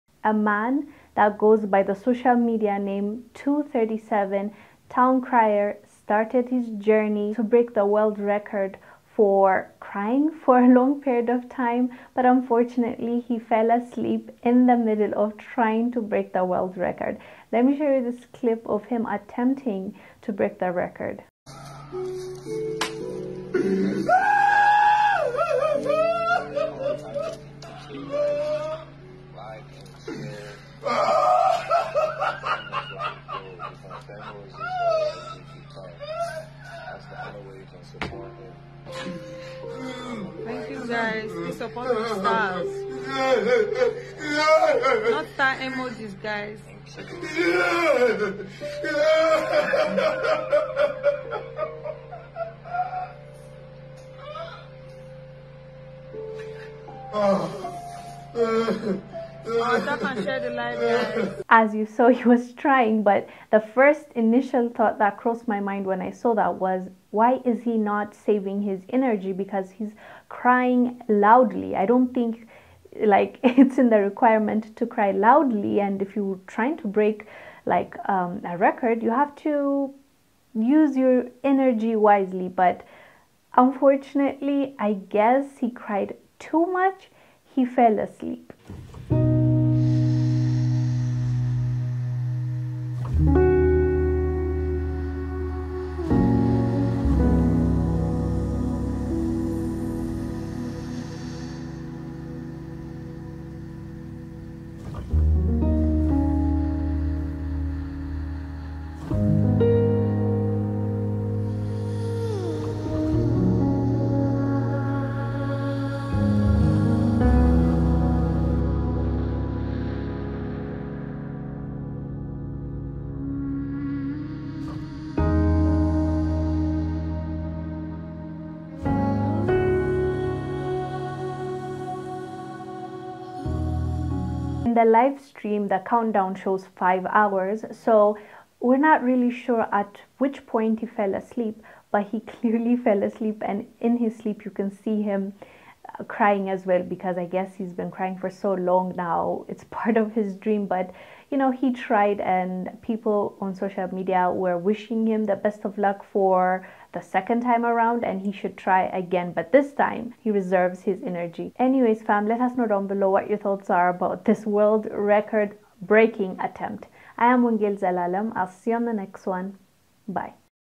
Man Falls Asleep Attempting To Break A World Record For Longest Time Crying
Wongel Zelalem reports on man falling asleep attempting to break a world record for longest time crying.